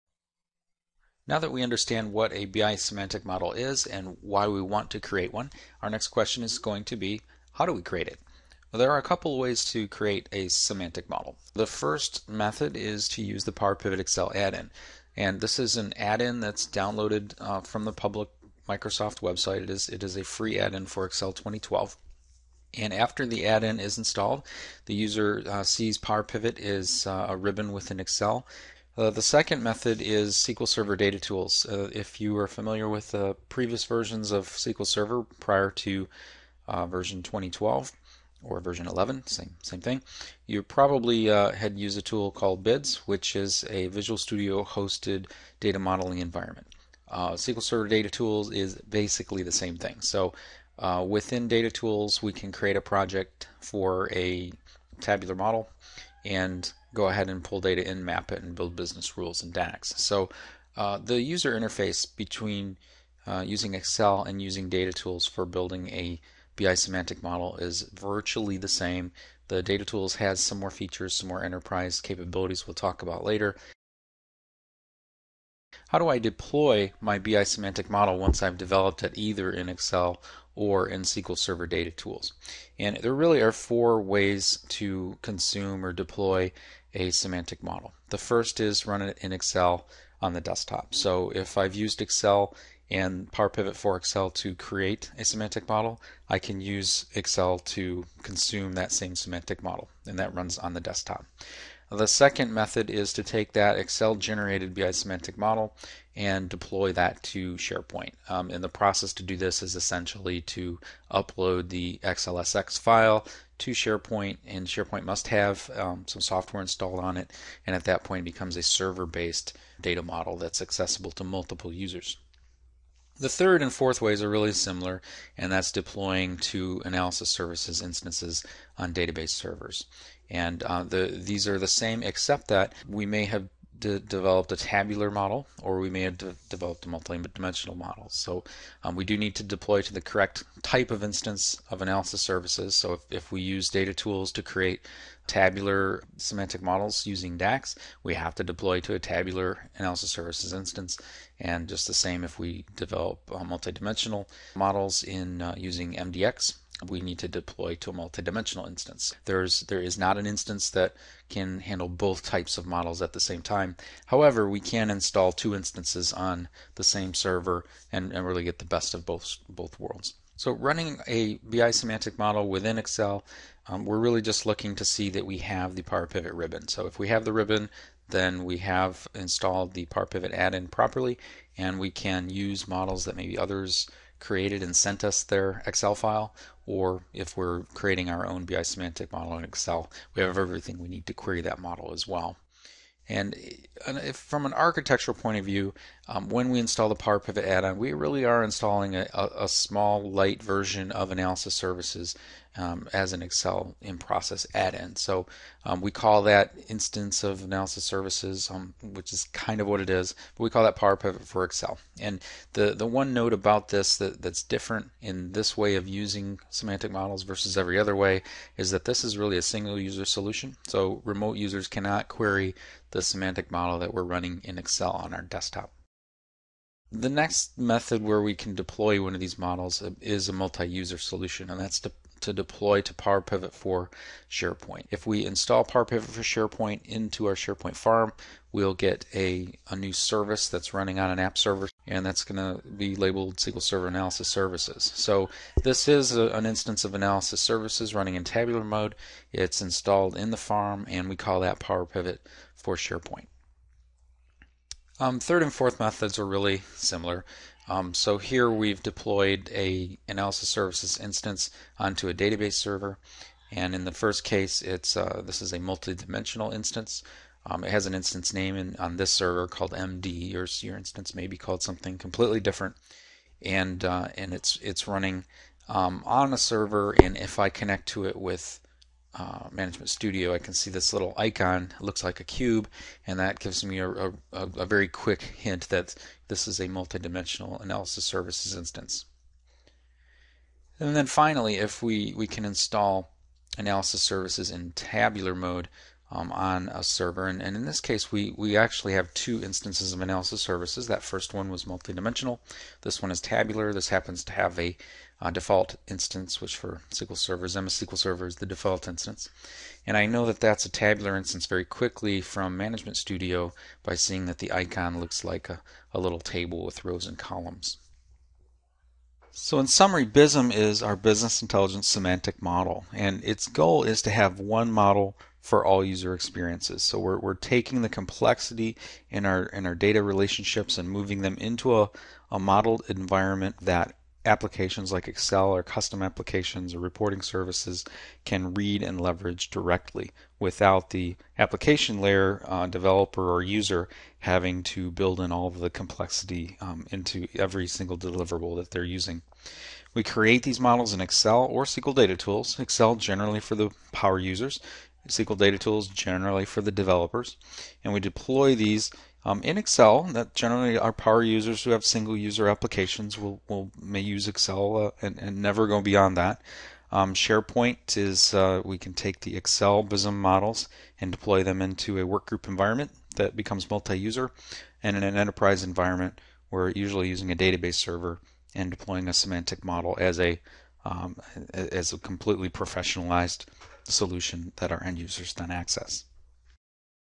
Introduction to BISM (2 of 2)
What technologies are used to create and deploy BI Semantic Models